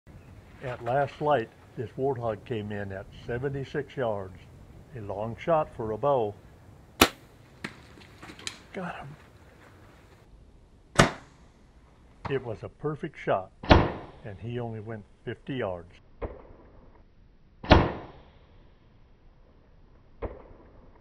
Warthog at 76 yards, with a Bow!
It was the last day, no the last hour, of a hunt in Africa when a warthog came into the waterhole that I was sitting on. He was at 76 yards! For me that is a very long shot with a bow and arrow! It was a perfect hit, through both lungs and the large male warthog only made it 50 yards before going down.